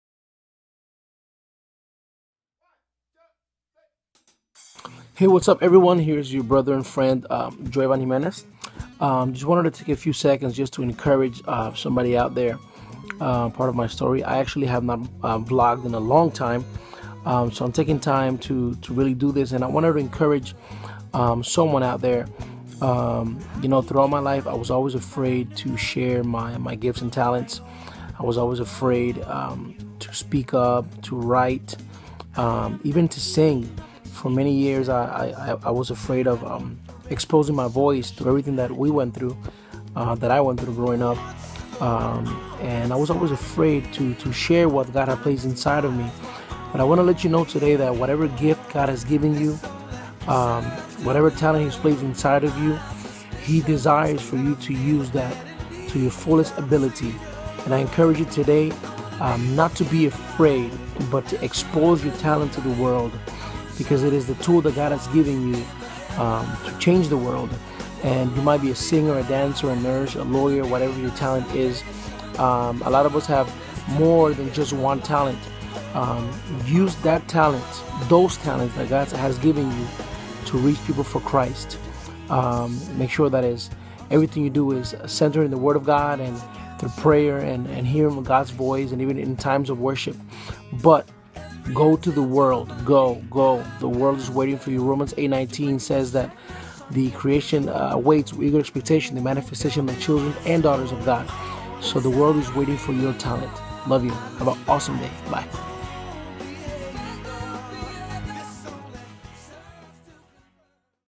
Joivan Jimenez VLOG "Go"
Joivan shares an inspirational story about our talents.